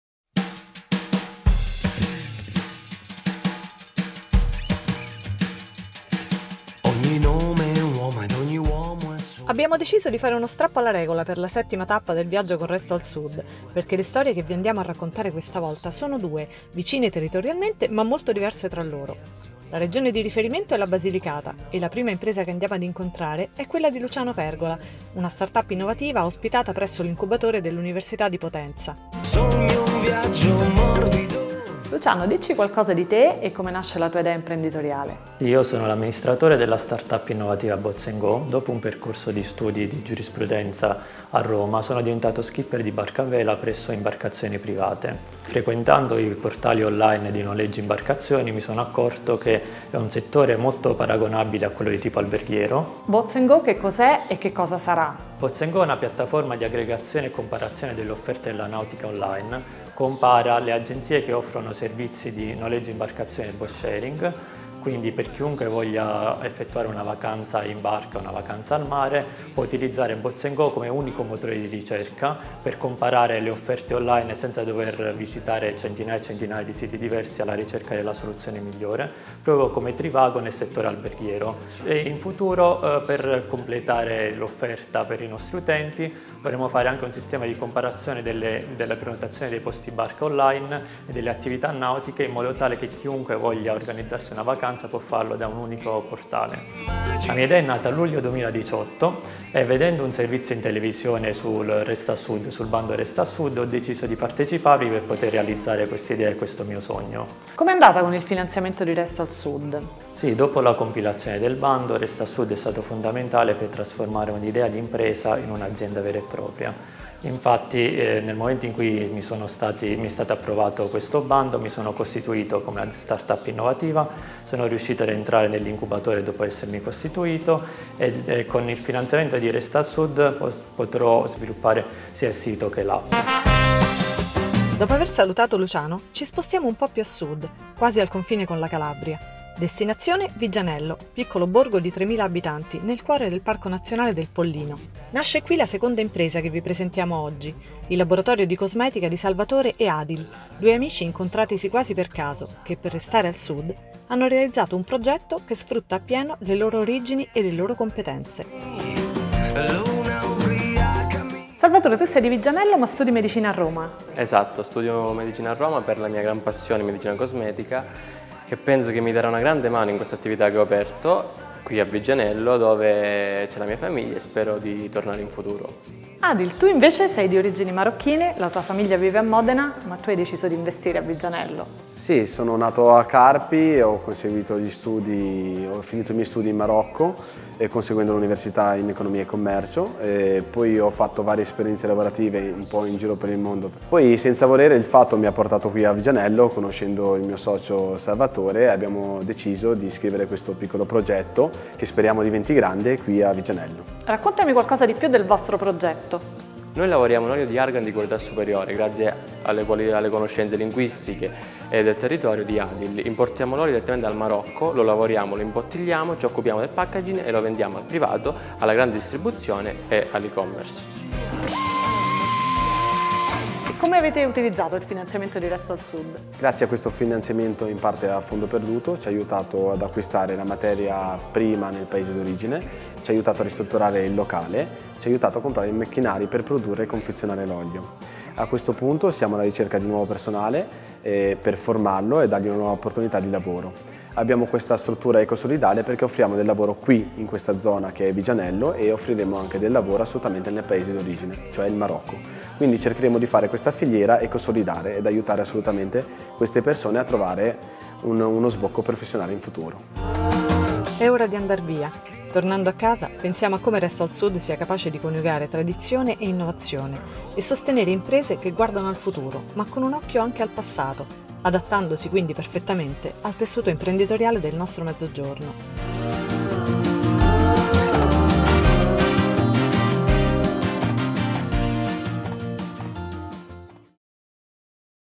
In viaggio con Resto al Sud - 7 tappa Potenza
Facciamo uno strappo alla regola per la settima tappa del viaggio con Resto al sud, perchè le storie che vi andiamo raccontare sono due, vicine territorialmente, ma molto diverse tra loro.

Siamo in Basilicata e la prima impresa che incontriamo è BoatsandGo di Luciano Pergola, una startup innovativa ospitata  nell'incubatore di Sviluppo Basilicata, all'interno dell’Università di Potenza.
Poi ci spostiamo un po’ più a sud, quasi al confine con la Calabria, a Viggianello, piccolo borgo di 3000 abitanti, nel cuore del Parco nazionale del Pollino.

Nasce qui la seconda impresa nata con Resto al Sud, Hyris Lab, il laboratorio di cosmetica di Salvatore Anastasio e Adil Rafia.